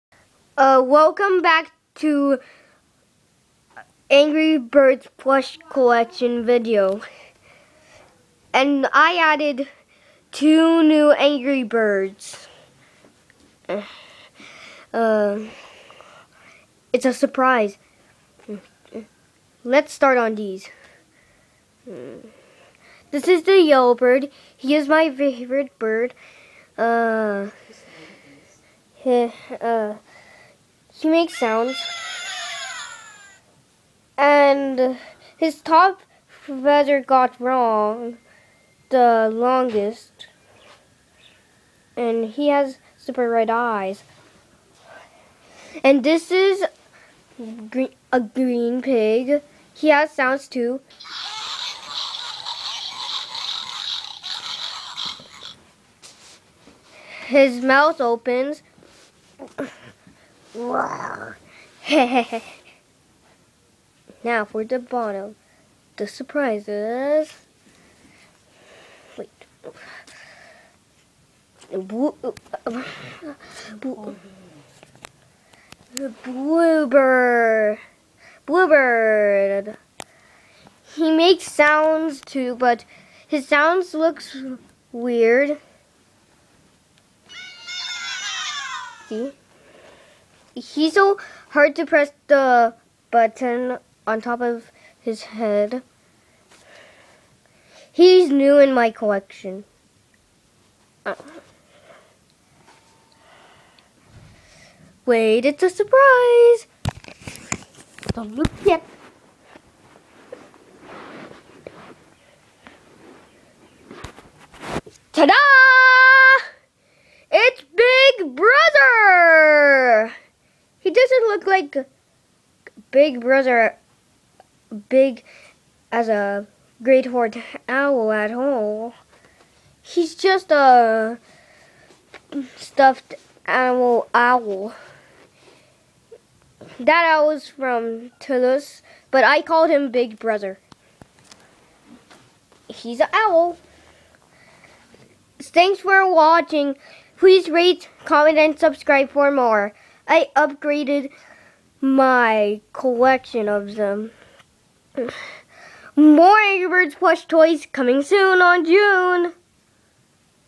Angry birds plush toy update
I lost my handmade orange bird.
I traded my red bird.